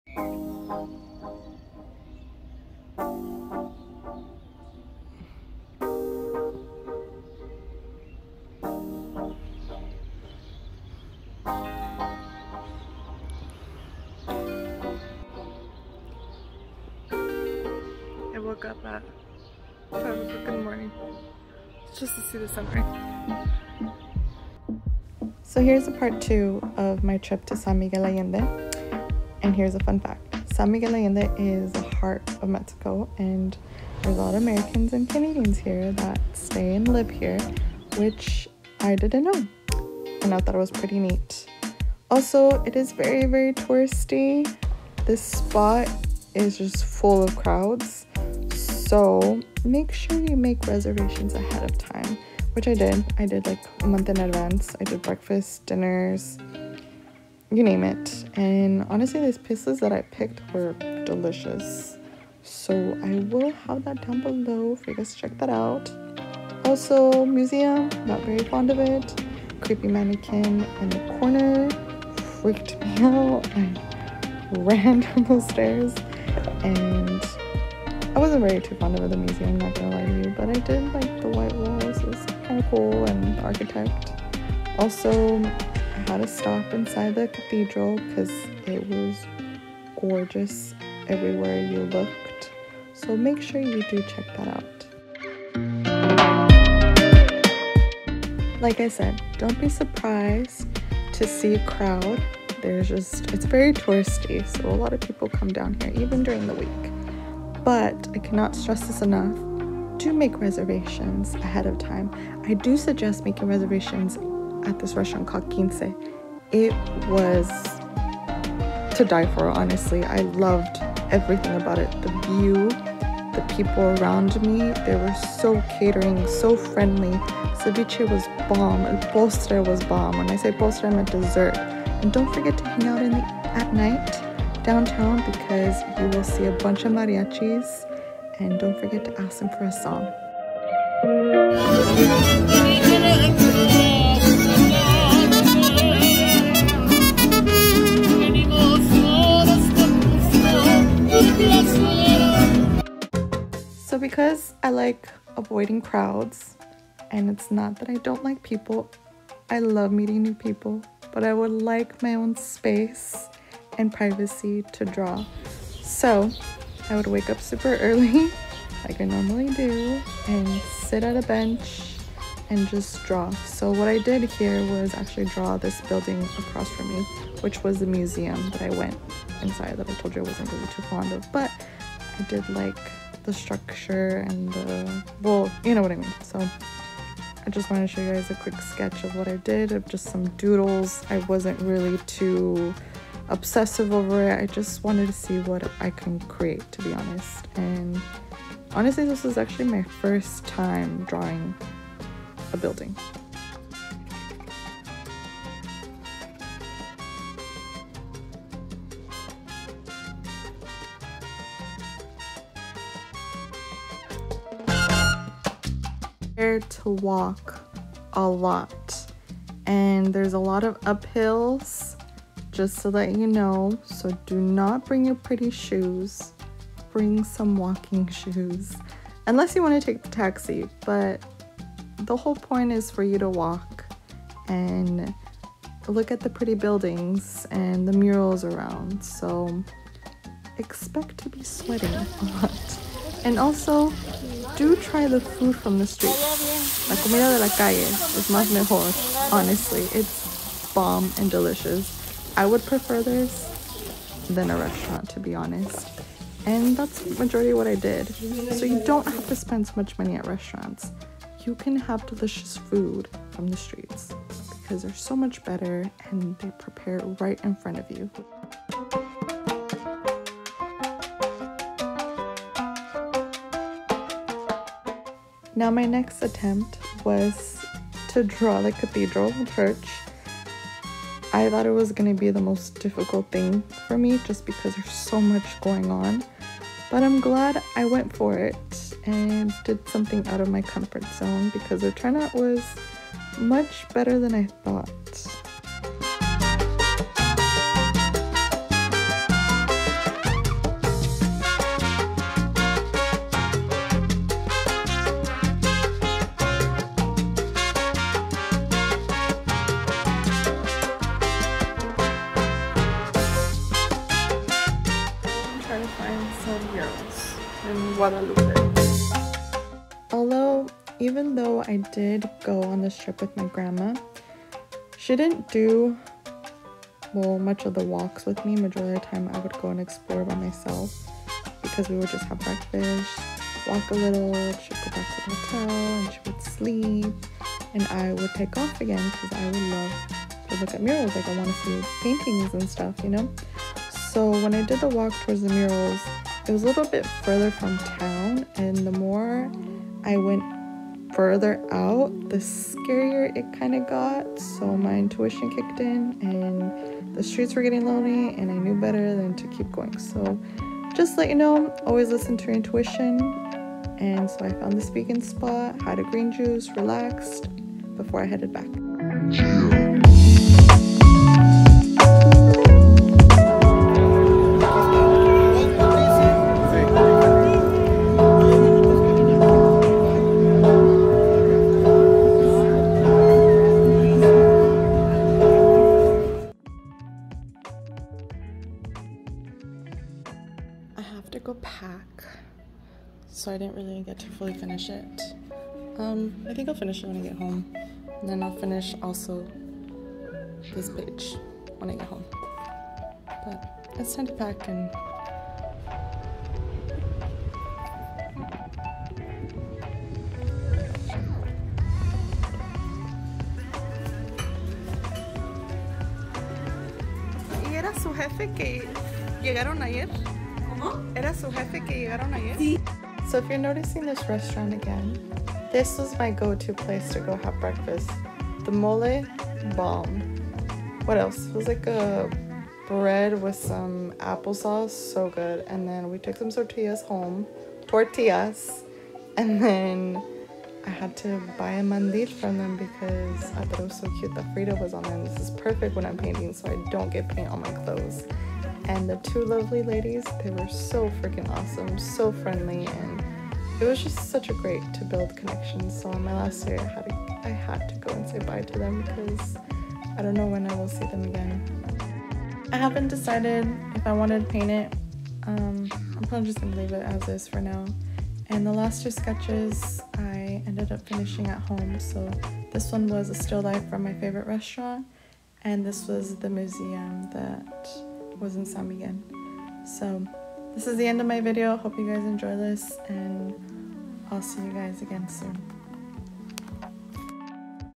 San Miguel Allende Part Two // Nani // Art Vlog
This video is part two of my travel to San Miguel Allende, Mexico. Also, continuing sketching and painting in my sketchbook.

Food vendors: Around the markets:
Mercado Ignacio Ramirez

Vegan Spot:
La Raiz de la Tierra

Desert:
Chocolates y Churros San Agustin

My Favorite Go-to Spot for Coffe and Breakfast:
Cafe Casa Mexico